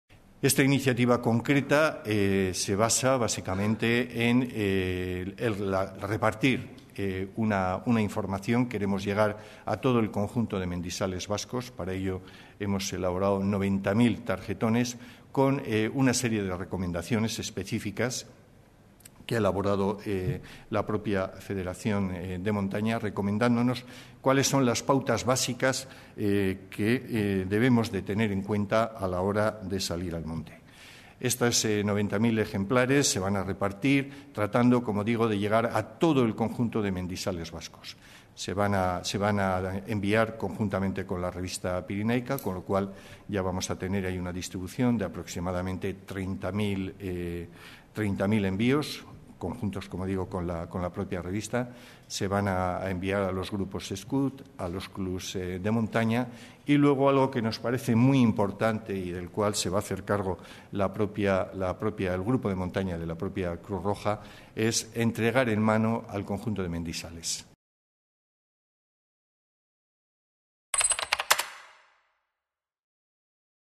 El Gobierno Vasco impulsa una campaña de prevención dirigida a los amantes de la montaña
Más de 90.000 folletos de carácter preventivo serán repartidos entre el colectivo mendizale. El Viceconsejero de Interior, Raúl Fernández de Arroiabe, considera "muy importante" que gran parte de ellos se entreguen "en mano" a pie de monte

El Departamento de Interior del Gobierno vasco, a través de la Dirección de Atención de Emergencias y Meteorología, pondrá en marcha a partir del próximo 20 de mayo una campaña de prevención en montaña consistente en la difusión de más de 90.000 folletos de carácter didáctico y preventivo que serán distribuidos entre el colectivo mendizale muchos de ellos en las faldas de los principales montes de la CAV y en pruebas deportivas de carácter multitudinario